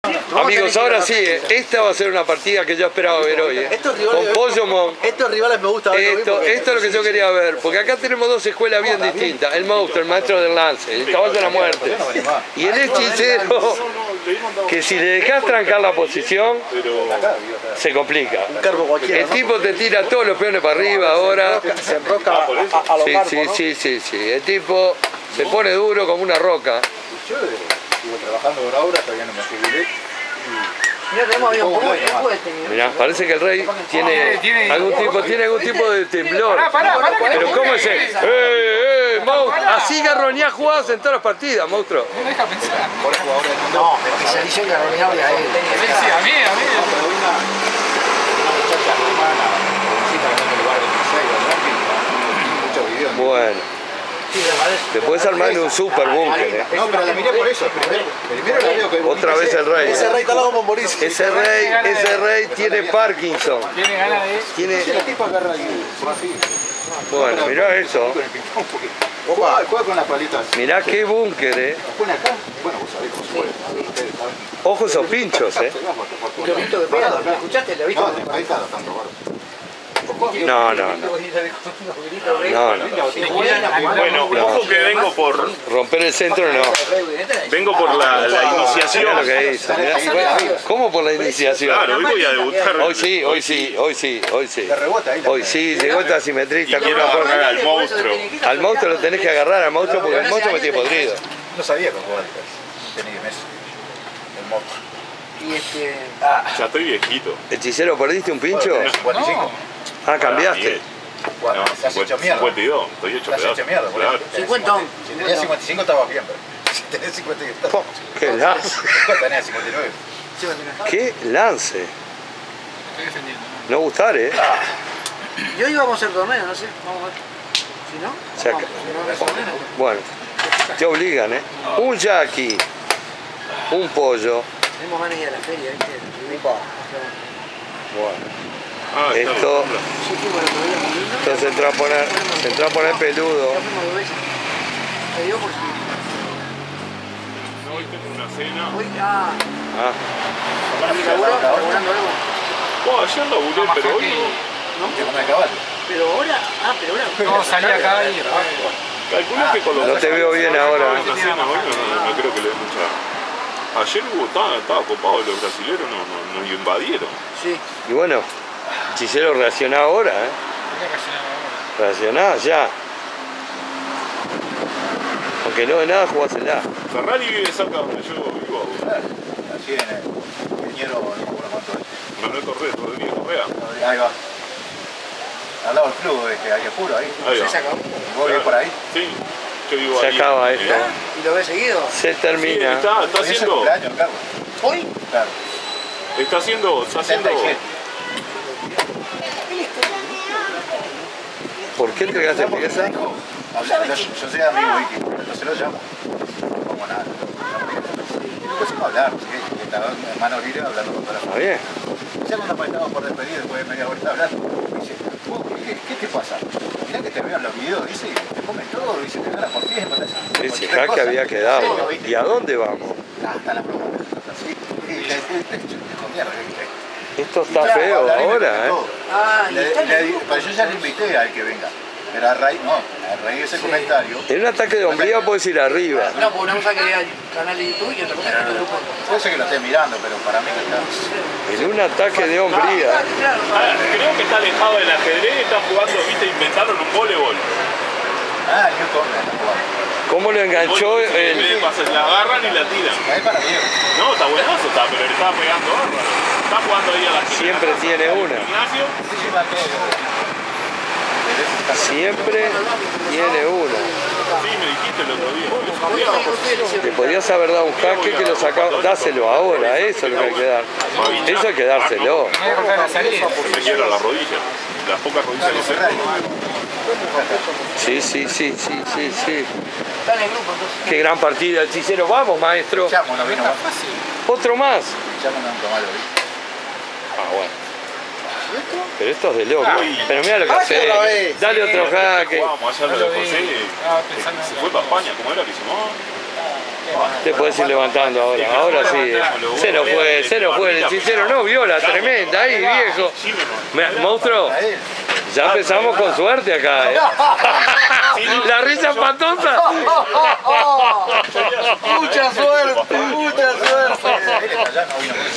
ajedrez callejero (con un poco de suerte)
NUEVO , Amigos esta partida la esperaba ansioso, aca tenía que darse un buen juego, el monstruo el maestro del lance, se encuentra con el hechicero muy sólido con su bunker y jugando más rápido se manda una partida de las cerradas con unos pinchos de miedo, pierde uno por un burro y bueno aca la posición esta como un campo minado, hay lances por todas partes, el monstruo se come un jaky en la 8va que le afloja las tuercas, entrega un burro el hechicero por pegar un jaky y dejar un hipocampo instalado y aca se equivoca y en grande, en vez de seguir dando jaky cuelga el burro y se diluye todo, se nos va el hechicero escuchando la risa del monstruo que yallegó con suerte y se salvó por un pelo, Bar Hispano, Montevideo, Uruguay.